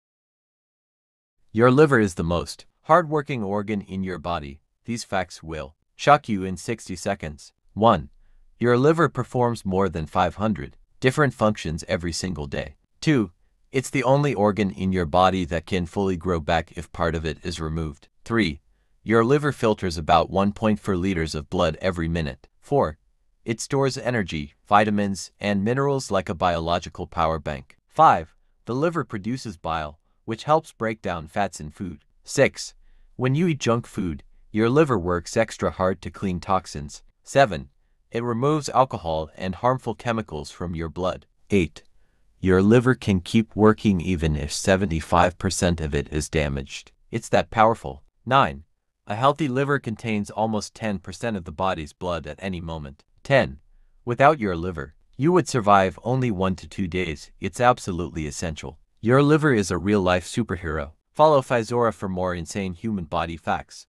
“10 Shocking Facts About Your Liver!”
Your liver is the most powerful detox machine ever created! From performing over 500 functions to regenerating itself — here are 10 amazing liver facts in just 60 seconds.